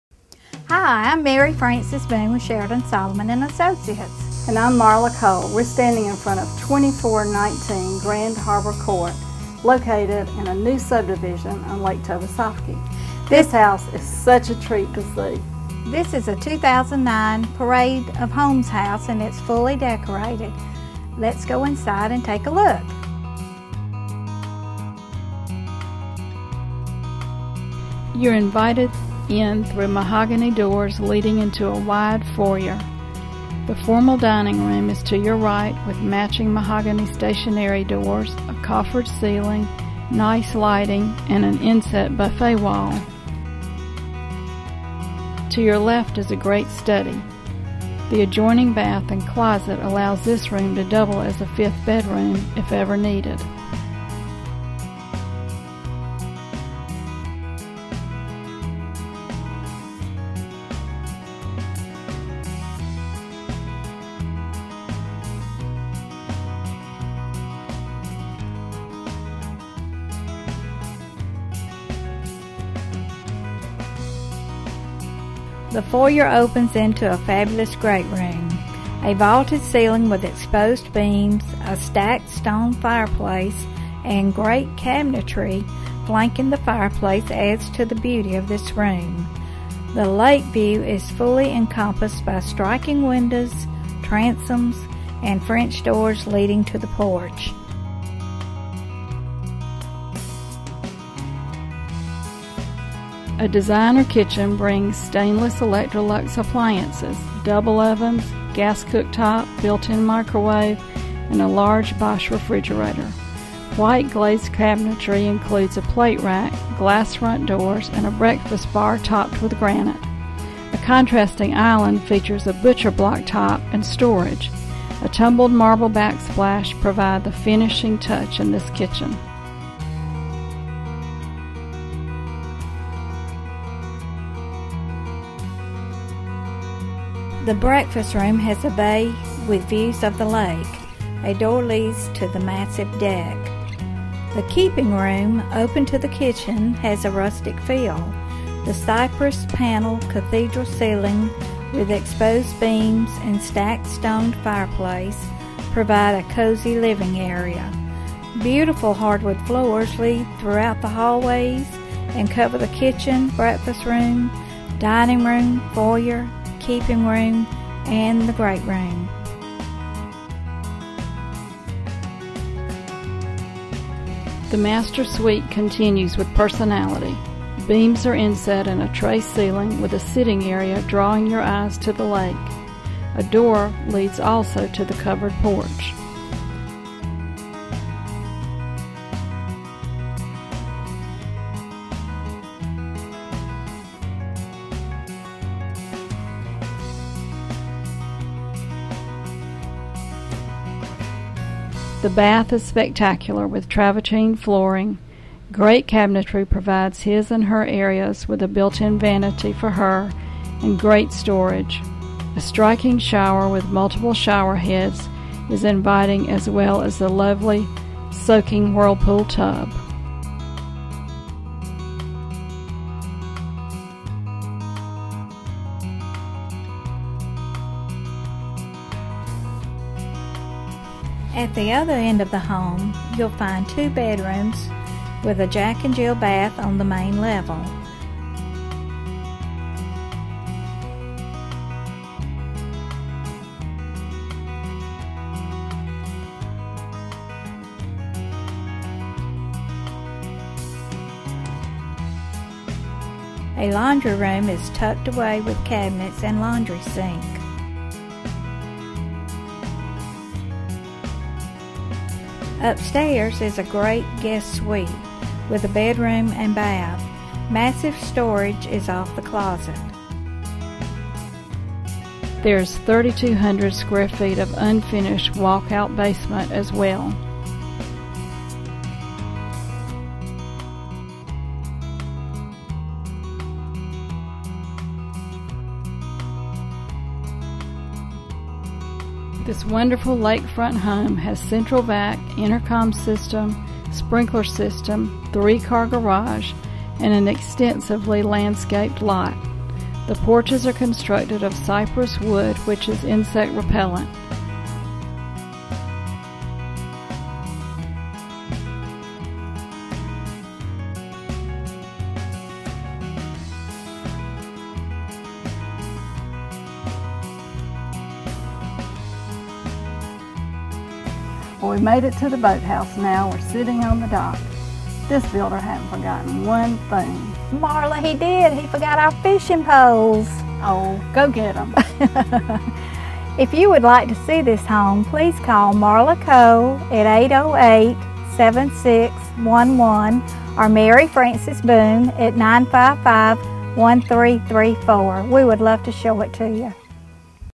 2419 Grand Harbor Court Lizella, GA 31052
Fabulous home on lake with amazing views. 4 or 5 BRs. Huge unfinished basement with endless possibilities. New development on Lake Tobesofkee - Grand Harbor - All "bells and whistles" - quality built with attention to detail